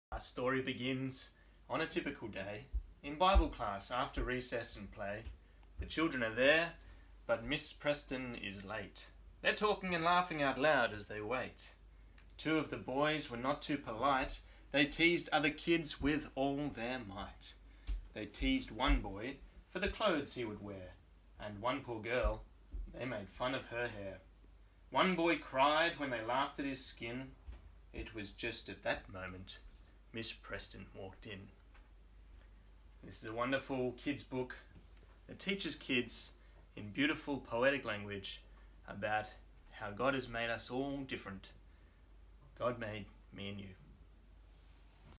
God Made Me and You: Reformers Book Review
Tom reads a portion of Shai Linne's children's book "God Made Me and You" from New Growth Press.